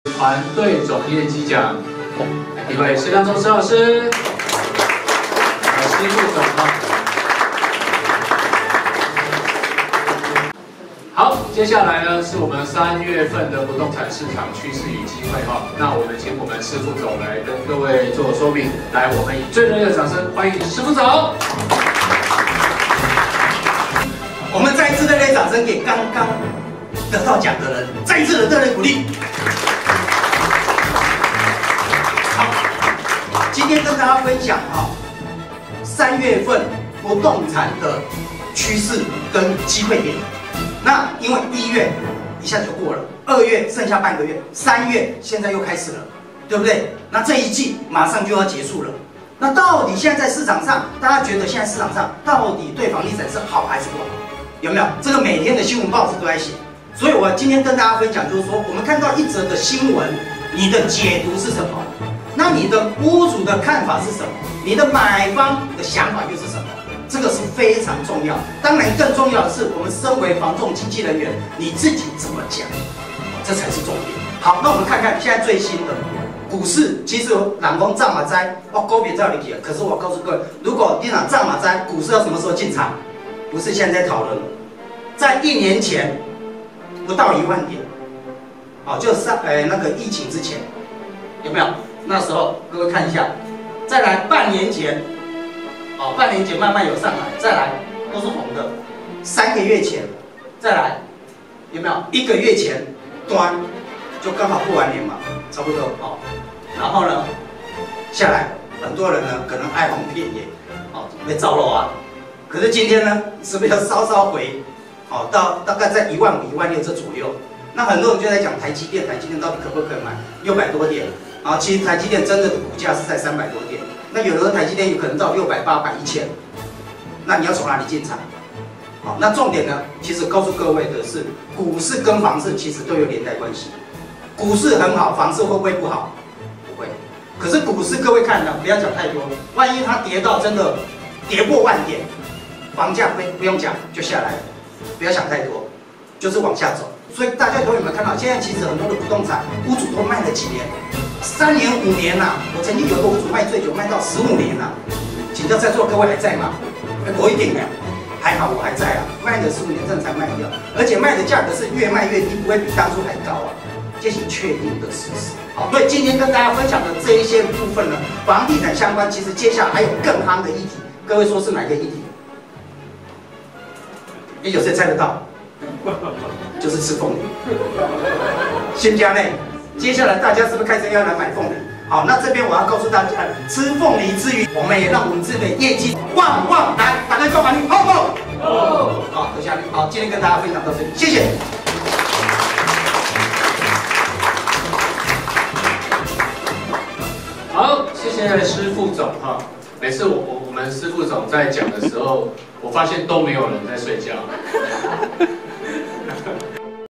2021年3月份 不動產市場趨勢與機會 施亮州執行副總 分享
2021年3月份 不動產市場趨勢與機會 施亮州執行副總 分享
2021年 ERA新經濟新平台 已是趨勢新顯學！
歡迎共襄盛舉 2021-3-8